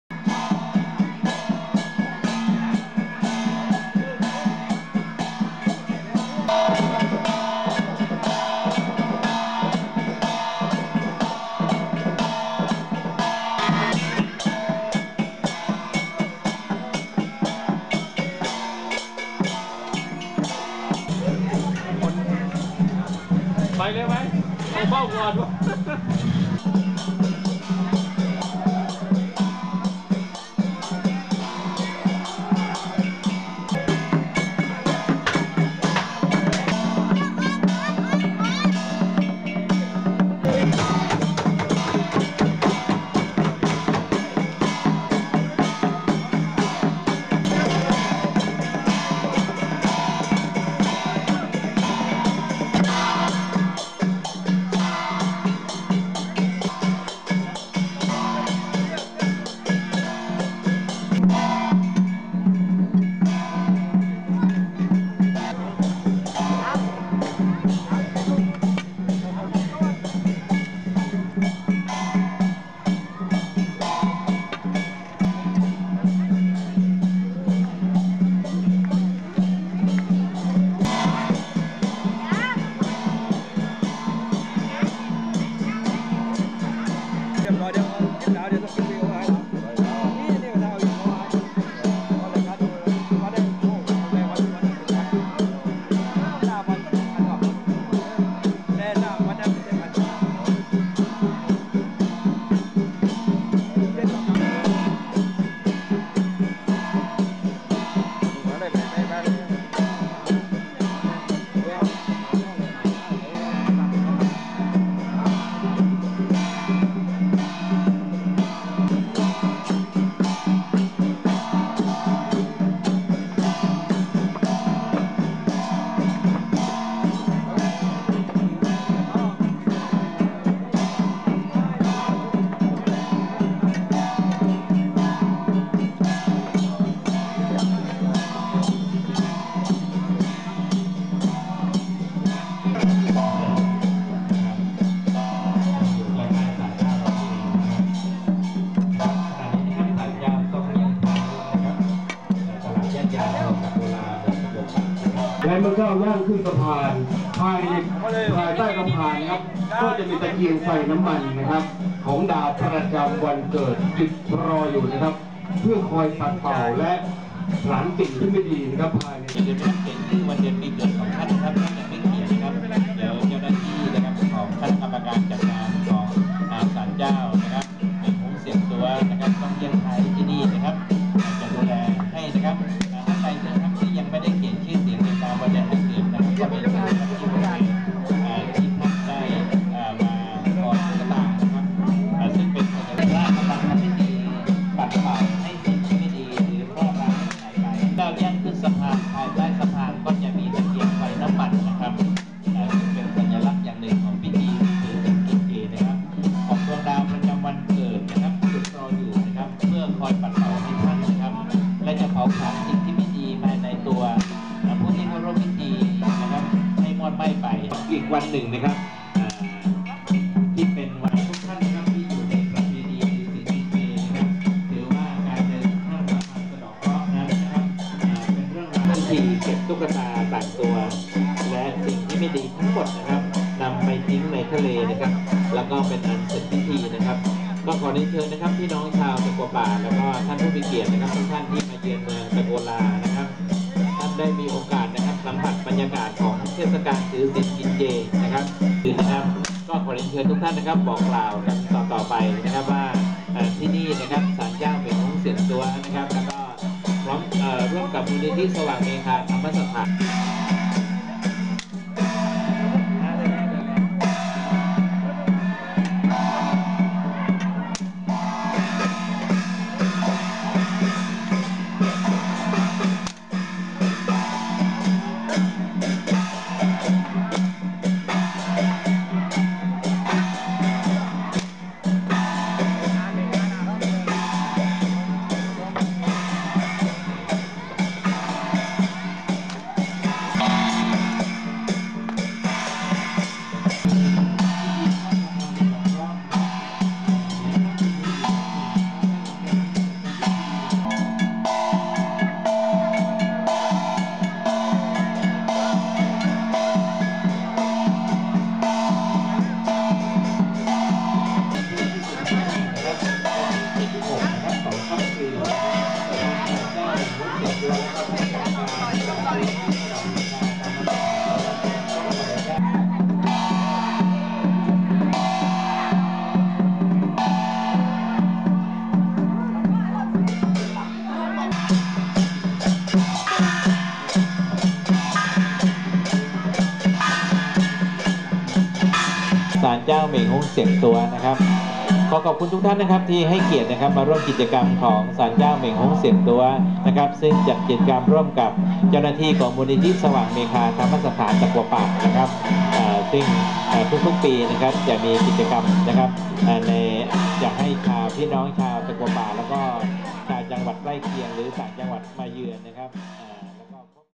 โก๊ยห่าน
พิธีโก๊ยห่าน
(ข้ามสะพานสะเดาะเคราะห์)
มูลนิธิสว่างเมฆาธรรมสถาน ตะกั่วป่า จ.พังงา

เป็นอีกหนึ่งพิธีที่มีความสำคัญปฏิบัติกันมาตั้งแต่สมัยโบราณ
มีความเชื่อและความศรัทธาว่า
หากให้การถือศีลกินผักสมบูรณ์แบบครบถ้วนควรเข้าร่วมพิธี
ผู้เข้าร่วมต้องมี “โต้ยซี้น” หรือตุ๊กตาต่างตัวเขียนชื่อวันเดือน ปี เกิด แล้วเดินข้ามสะพาน
ในขณะที่พระทำพิธีปัดเป่าให้สิ่งไม่ดีออกไปเหลือไว้แต่สิ่งที่ดี
มีความสุข มีโชคดี มีลาภ ตลอดปี
ก่อนขึ้นสะพานม้าทรงประทับตราที่ด้านหลังของเสื้อที่ใส่เรียกว่า ค้ำยิ่น
ใต้สะพานมีตะเกียงไฟน้ำมันของดาวประจำวันเกิดจุดรออยู่ด้านล่าง
เพื่อปัดเป่าเผาผลาญสิ่งที่ไม่ดีภายในตัวผู้เข้าร่วมพิธีฯให้มอดไหม้ไป ครั้นเดินลงจากสะพานจะถูกล้างตัวอีกครั้ง
โดยไฟโก้ยเช้งอิ๋ว ซึ่งเป็นไฟศักดิ์สิทธิ์
หลังจากนั้นพระทำการเก็บตุ๊กตาต่างตัวและสิ่งไม่ดีทั้งหลาย
นำไปทำพิธีทิ้งในทะเล
จึงเป็นอันเสร็จพิธีฯ

(ขอบคุณข้อมูลจาก หนังสือ เมืองตะกั่วป่าฯ รศ.สมโชติ อ๋องสกุล